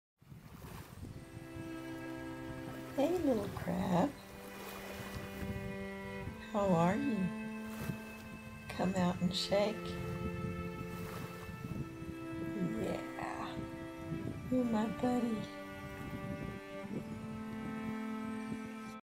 crab encounter
A close encounter with a hermit crab at the beach